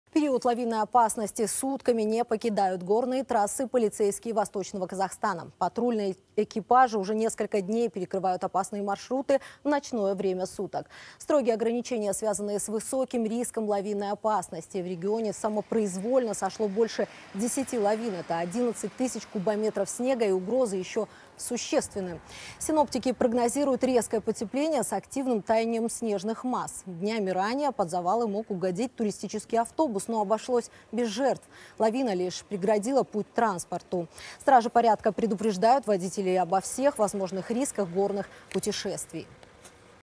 Полицейские ВКО в период лавинной опасности сутками не покидают горные трассы
В период лавинной опасности сутками не покидают горные трассы полицейские Восточного Казахстана. Дежурят у так называемых серпантинов. Патрульные экипажи уже несколько дней перекрывают опасные маршруты в ночное время суток. Строгие ограничения связаны с высоким риском лавинной опасности.